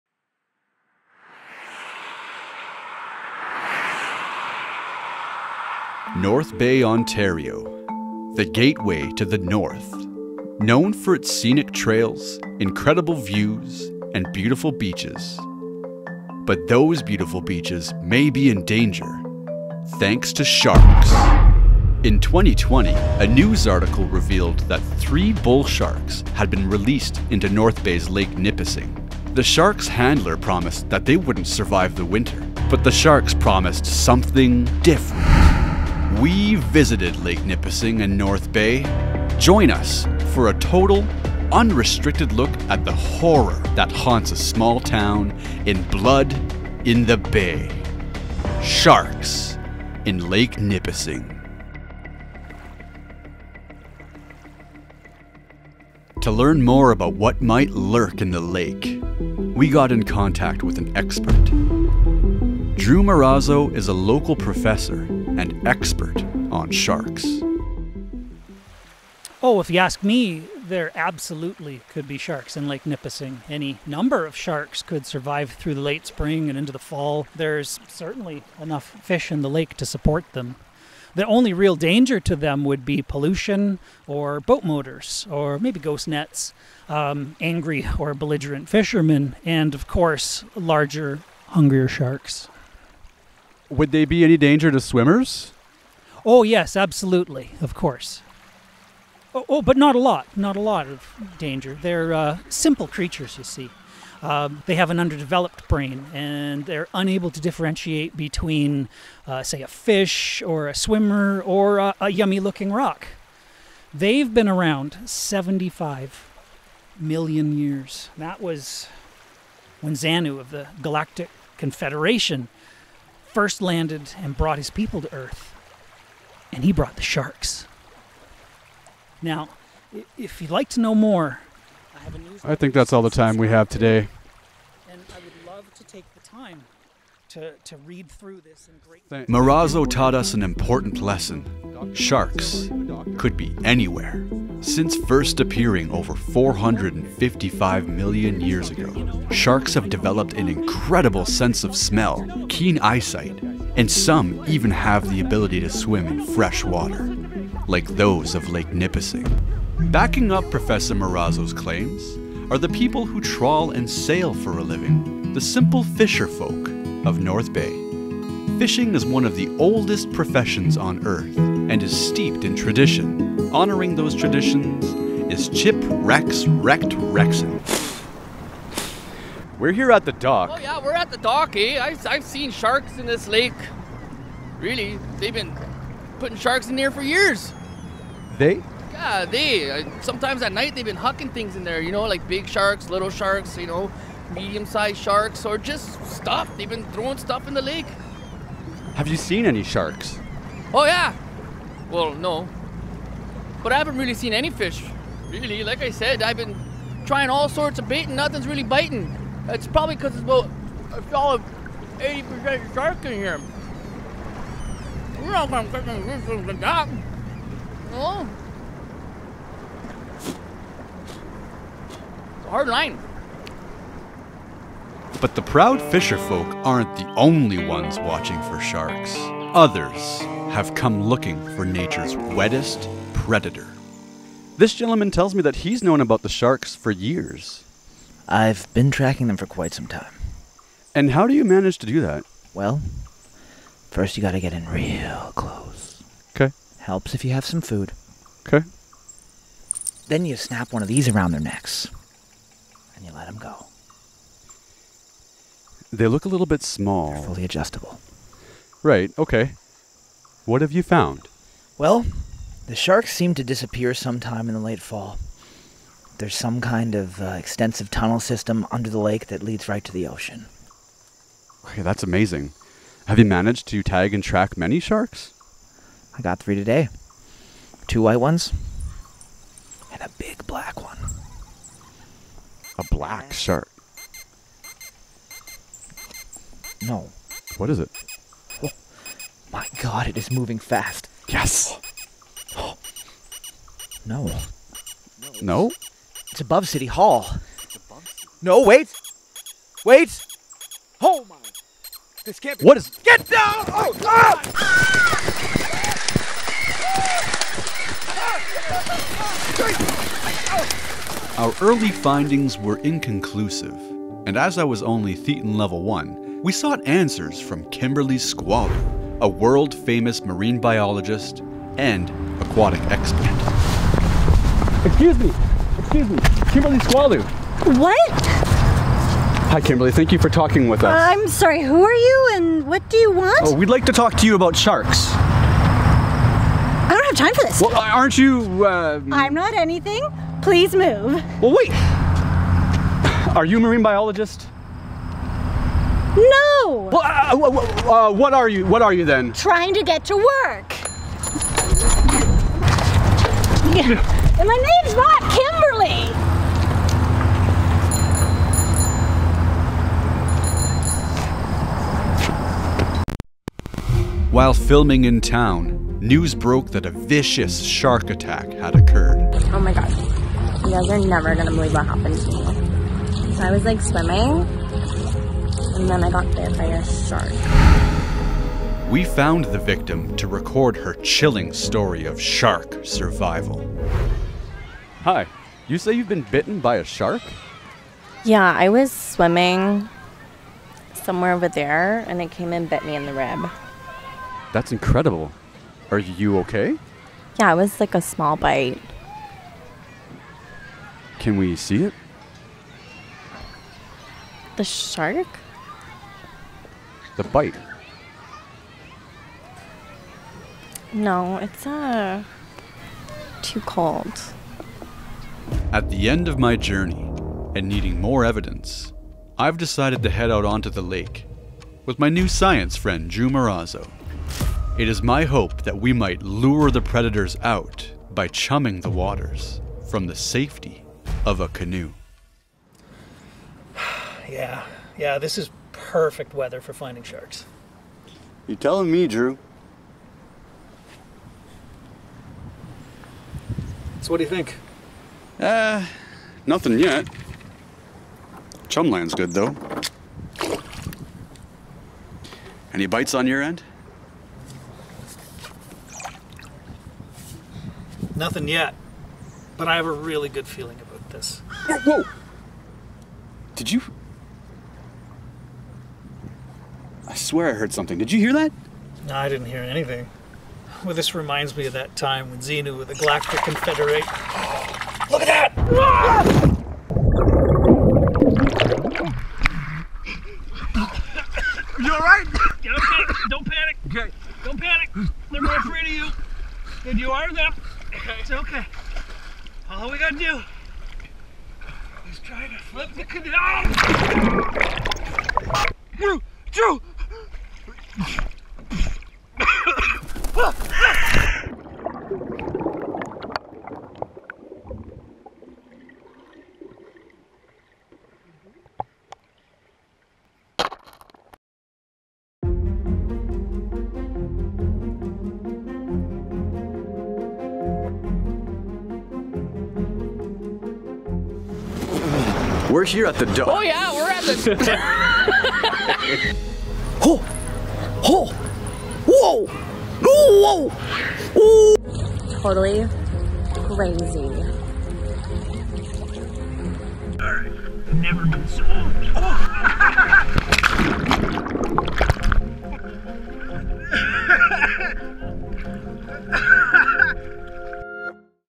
Blood in the Bay: Sharks in Lake Nipissing
In 2020, three bull sharks were released into Lake Nipissing. Now one man has set out to find them.